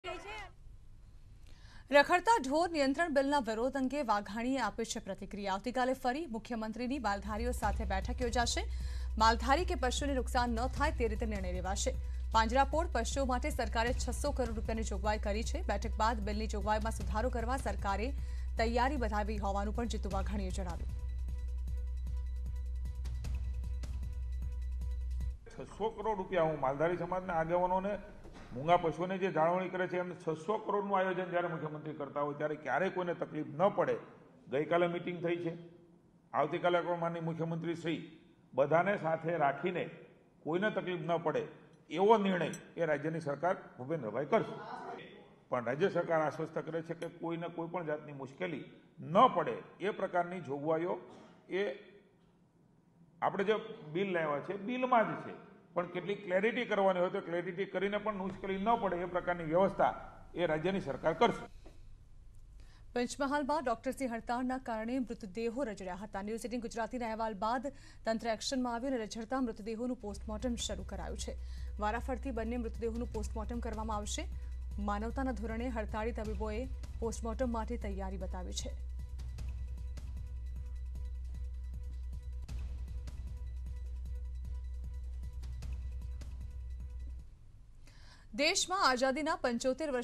Jitu Vaghani | આવતીકાલે CM ફરી યોજશે માલધારીઓ સાથે બેઠક : Vaghani | News18 Gujarati
રખડતા ઢોરના નિયંત્રણ Bill ના વિરોધ મામલે Vaghani એ આપી પ્રતિક્રિયા.આવતીકાલે ફરી યોજાશે CM ની માલધારીઓ સાથે બેઠક. સાથેજ Vaghani એ માલધારી કે પશુઓને નુકશાન ન થાય તેવી રીતે નિર્ણય લેવાશેની આપી ખાતરી.